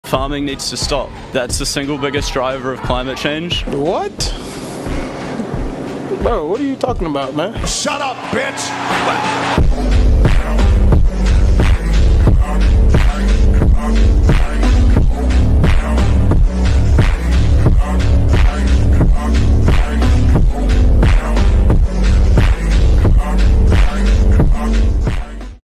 "Farming is Useless"
It is not easy to be a farmer you need a lot of knowledge and money to get into it. And even then you depend a lot on weather. So, show respect to everyone and always think before you speak! Stay strong King!

Here is my Instagram if you want help with something or have suggestions for content or just wants to chat